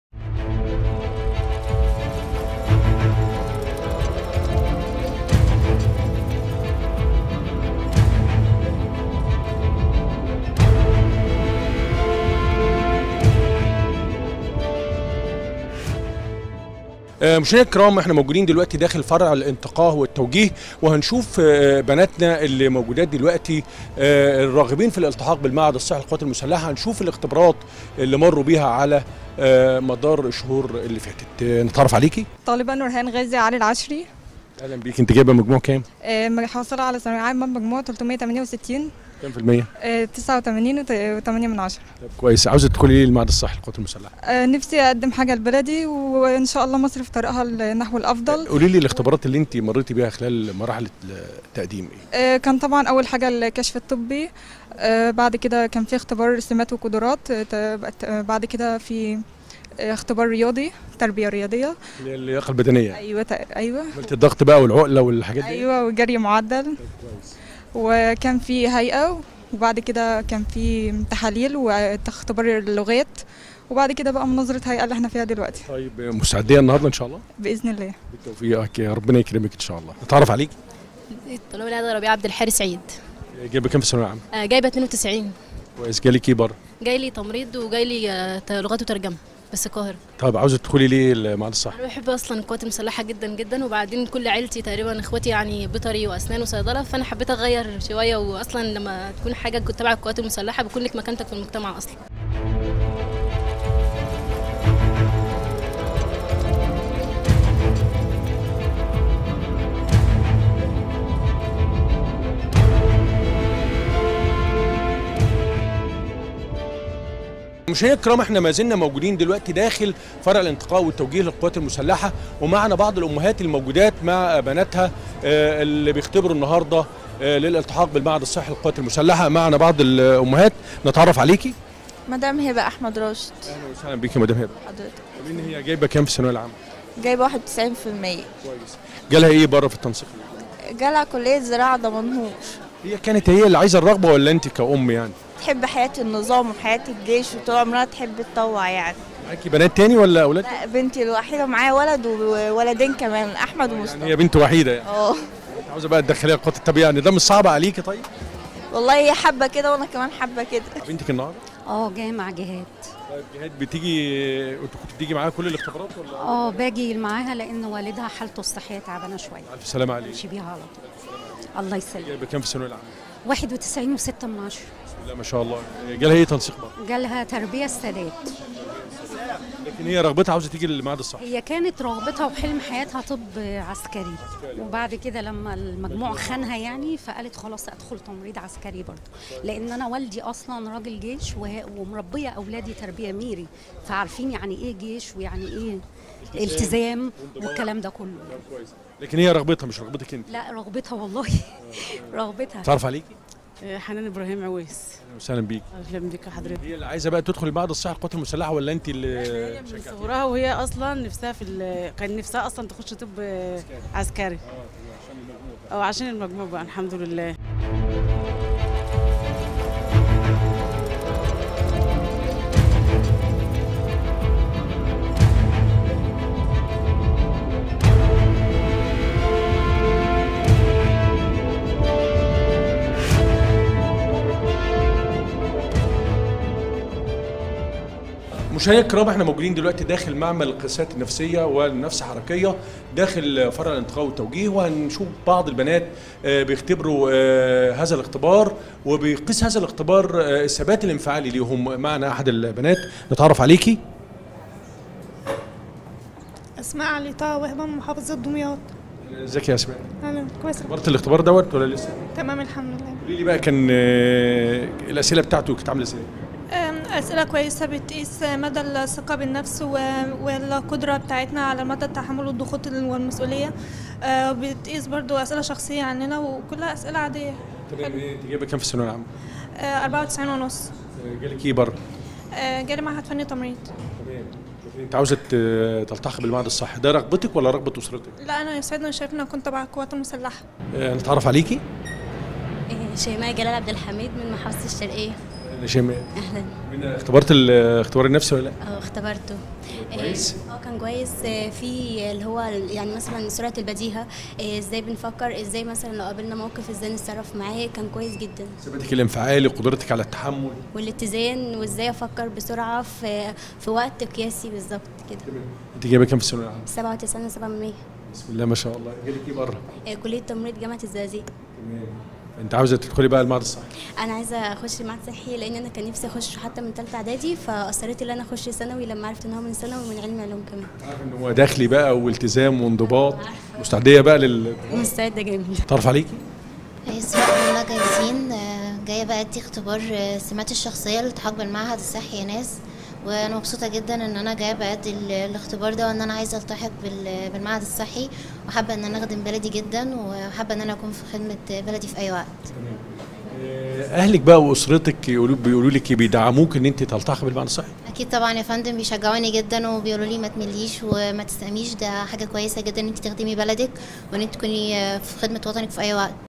لقاء خاص وحصري مع المتقدمات للالتحاق بـالمعهد الصحي للقوات المسلحة
درع_الوطن - لقاء خاص وحصري مع المتقدمات للالتحاق بـالمعهد الصحي للقوات المسلحة تقديم فولي ابو السعود